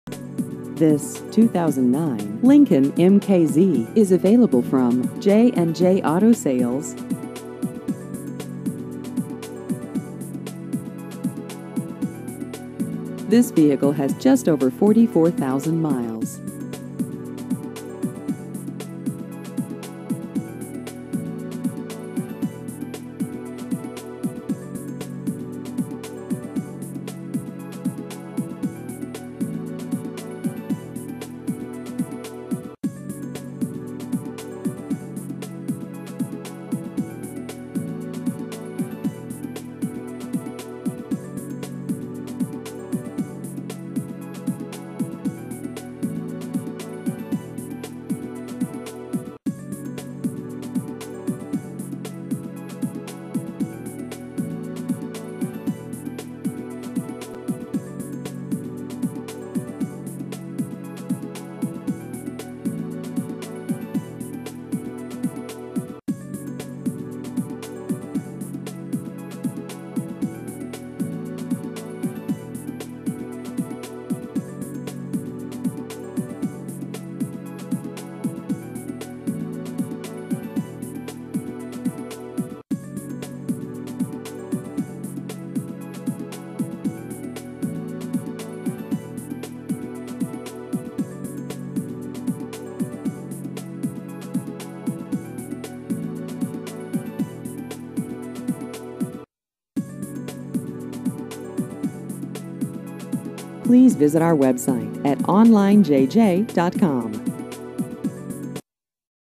2009 Lincoln MKZ Used Cars Troy NY
This 2009 Lincoln MKZ is available from J & J Auto Sales.